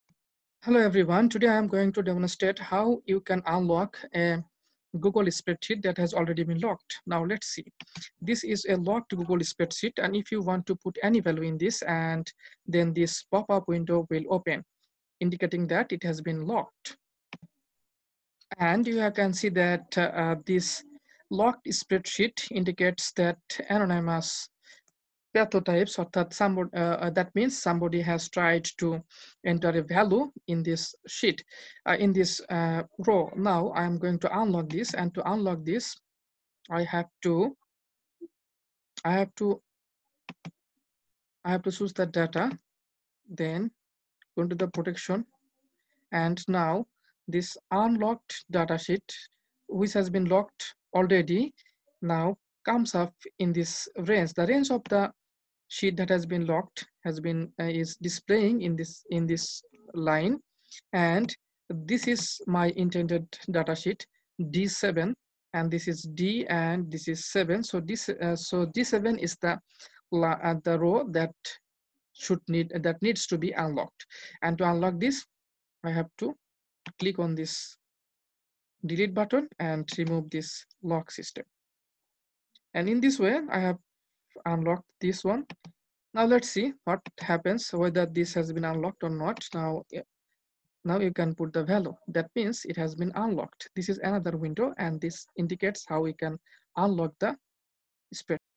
How to unlock Google spread sheet.? (Unlock protected google spreadsheet)
How to unlock Google spread sheet.(Unlock protected google spreadsheet), Remove  protection from sheet.